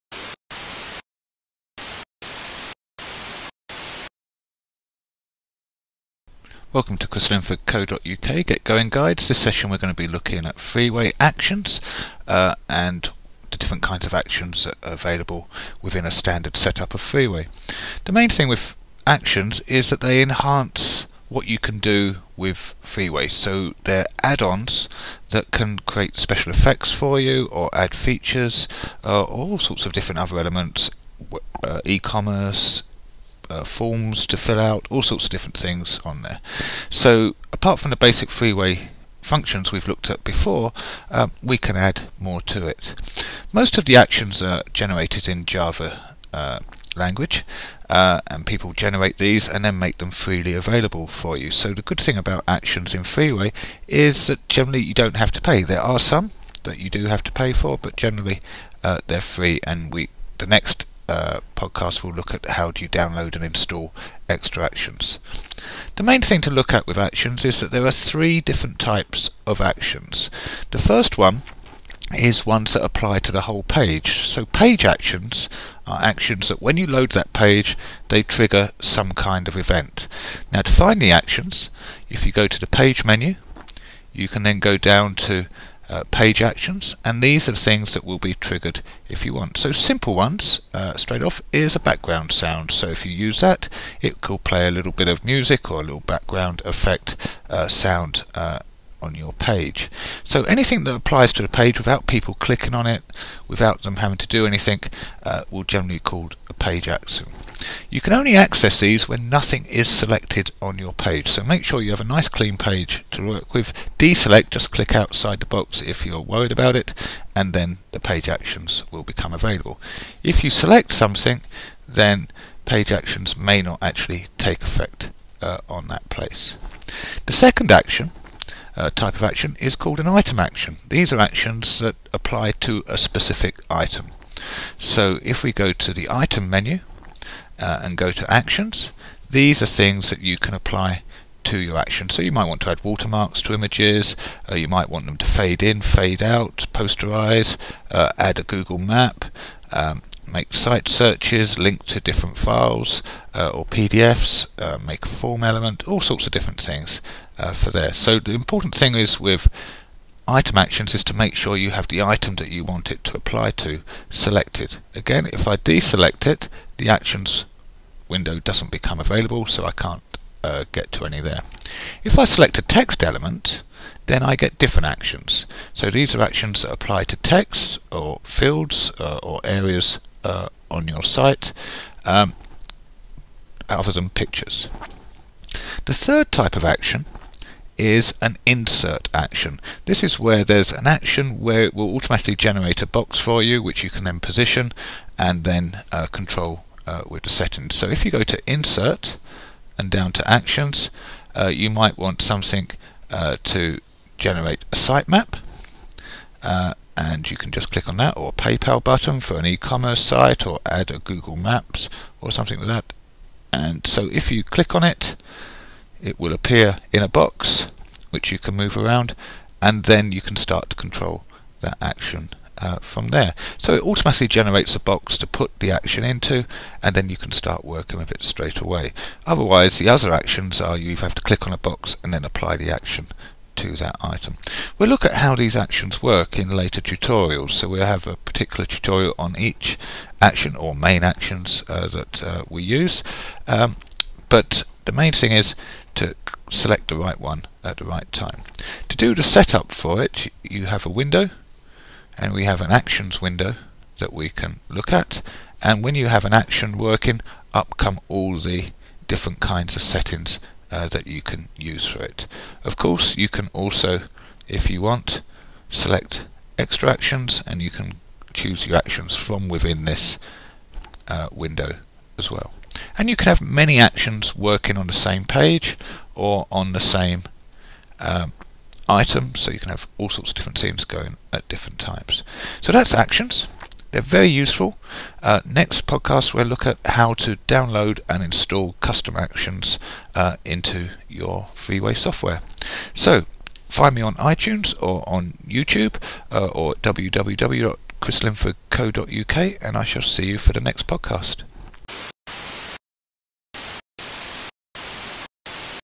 Freeway Lesson 32 - Freeway Actions
Freeway Actions allow you to add a wide range of content and features to your website. Most of these are free for you to download and use.